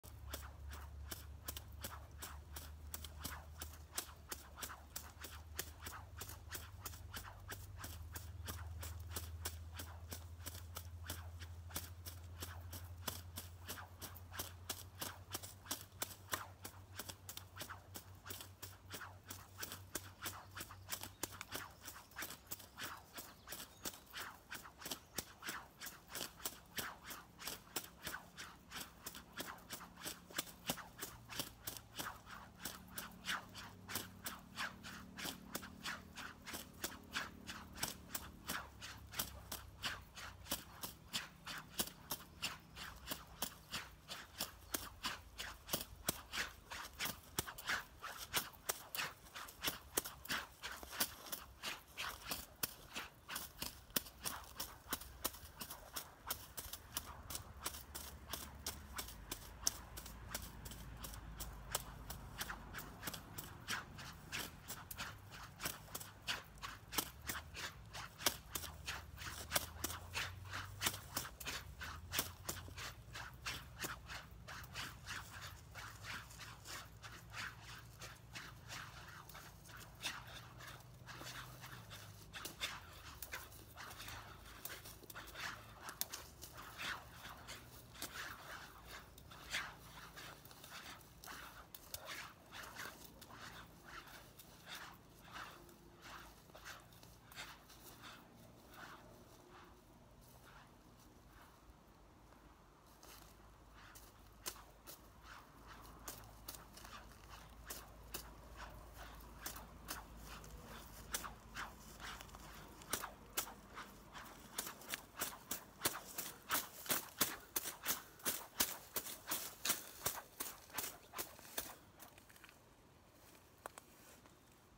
Holger Maik Mertin mit seinen Extra Langen Rods - Luft Sticks
Holger Maik Mertin - Rohema Endorser, Sound Design Artist und Multi Percussionist.
Es ist faszinierend, welche Sounds allein mit Gras und Luft enstehen können. FÜr Holger kann einfach jeder Raum zum Instrument werden. Er braucht dazu nur ein paar Tools - so wie seine extra langen und flexiblen Rohema Rods. Diese wurden speziell nach Holgers Wünschen und Ansprüchen entworfen und gebaut.

Mehr über Holger Maik Mertin